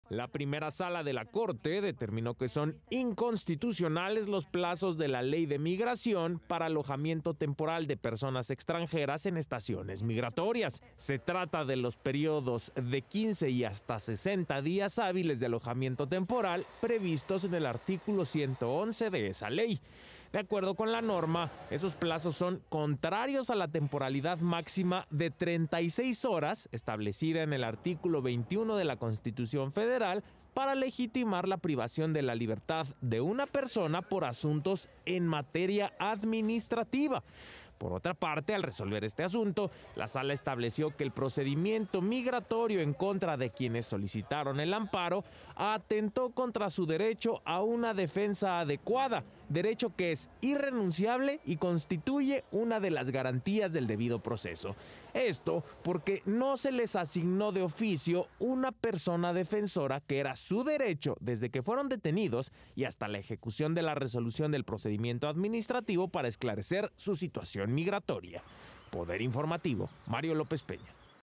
Inconstitucionales plazos de la Ley de Migración para alojamiento temporal de personas extranjeras
La Primera Sala de la Suprema Corte de Justicia de la Nación (SCJN) determinó que son inconstitucionales los plazos de la Ley de Migración para alojamiento temporal de personas extranjeras en estaciones migratorias.

De acuerdo con la ley, esos plazos son contrarios a la temporalidad máxima de treinta y seis horas.

Por otra parte, al resolver este amparo, la Sala estableció que el procedimiento migratorio en contra de quienes solicitaron el amparo atentó contra su derecho a una defensa adecuada, derecho que es irrenunciable y constituye una de las garantías del debido proceso.

Esto, porque se no les designó de oficio una persona defensora, que era su derecho desde que fueron detenidos y hasta la ejecución de la resolución del procedimiento administrativo para esclarecer su situación migratoria.

Con información de Mario López Peña.